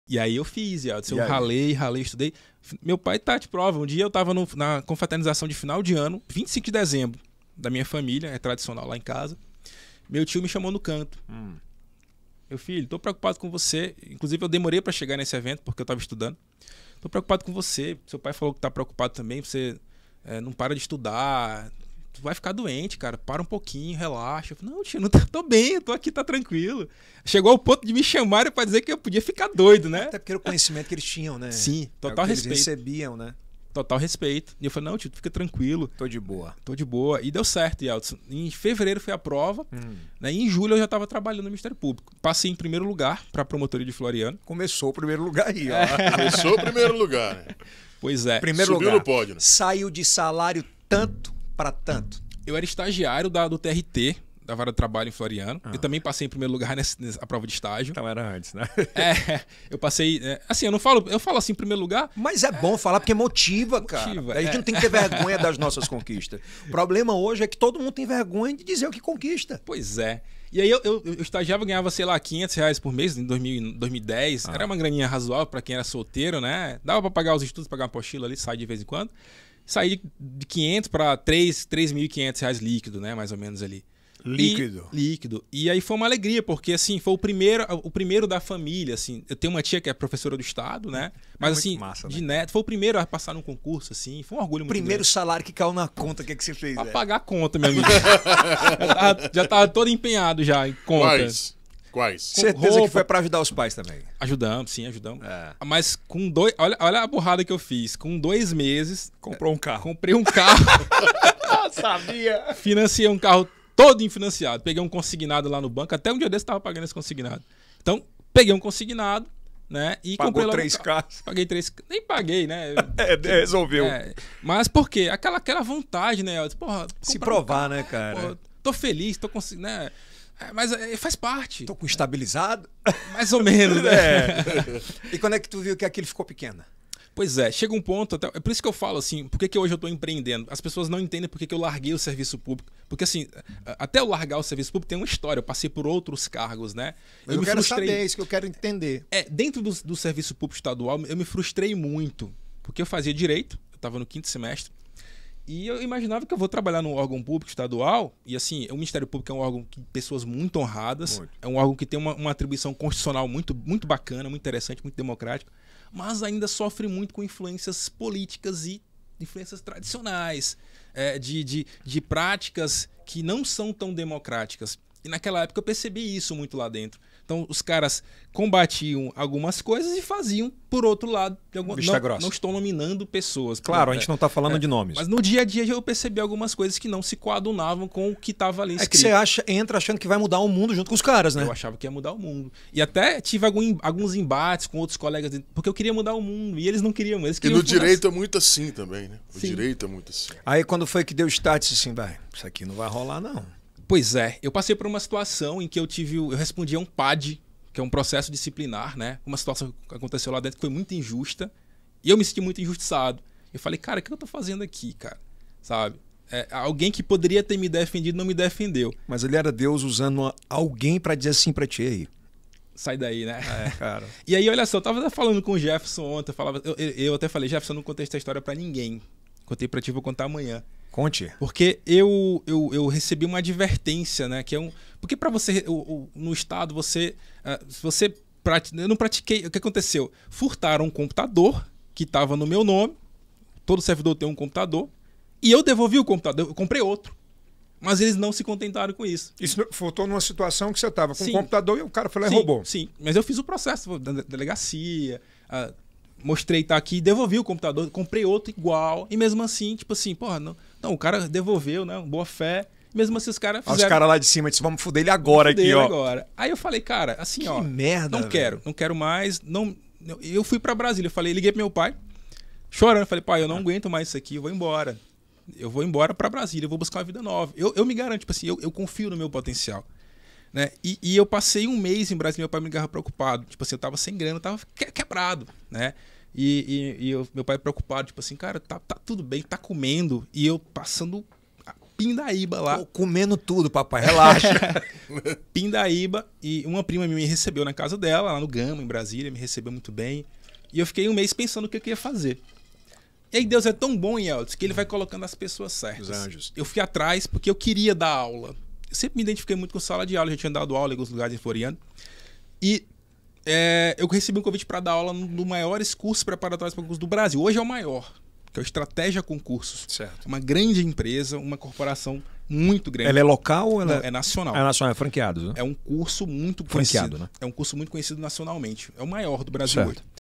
ABANDONO DO SERVIÇO PÚBLICO - PROFESSOR ARISTOCRATES CARVALHO
O convidado de hoje da nossa mesa de bar é o professor de redação e empresário, Aristócrates Carvalho. Natural de Floriano-PI, o professor é conhecido em todo o Brasil por vender cursos na Internet. No Ielcast, ele conta que se formou pelo Ensino de Jovens e Adultos (EJA), aprovação na Universidade Estadual do Piauí e no concurso no Ministério Público. Ele explicou ainda o motivo de ter abandonado o serviço público e a mudança para Santa Catarina! Uma história inspiradora. Piauiense que venceu na vida.

Ielcast, do Piauí para o Mundo!